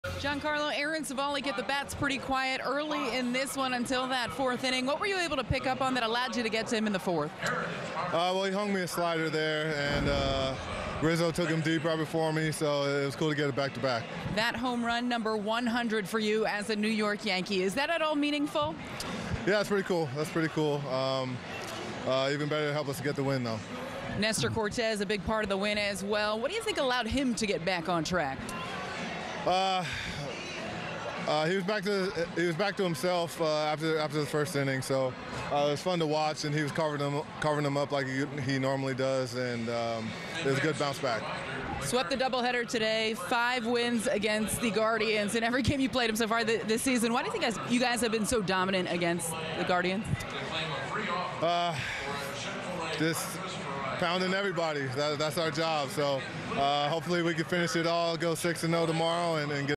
Giancarlo Stanton hits 100th HR as a Yankee
Giancarlo Stanton's 100th HR as a New York Yankee gave the team the lead for good in the doubleheader nightcap. Meredith Marakovits asks him about the milestone, the win, and going 5-0 so far this season vs. Cleveland.